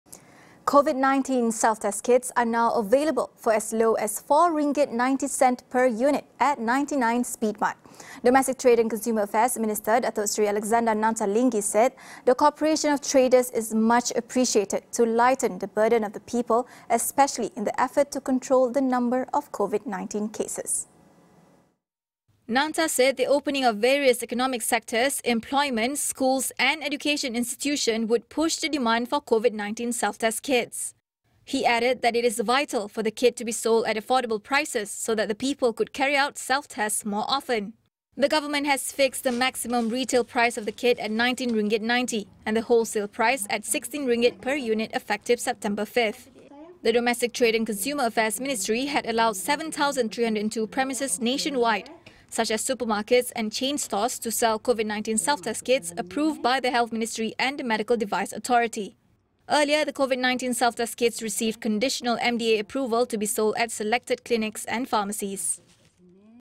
COVID-19 self-test kits now as low as RM4.90
Domestic Trade and Consumer Affairs Minister, Datuk Seri Alexander Nanta Linggi announced further reduction to Covid-19 self-test kits to lighten the burden of consumers.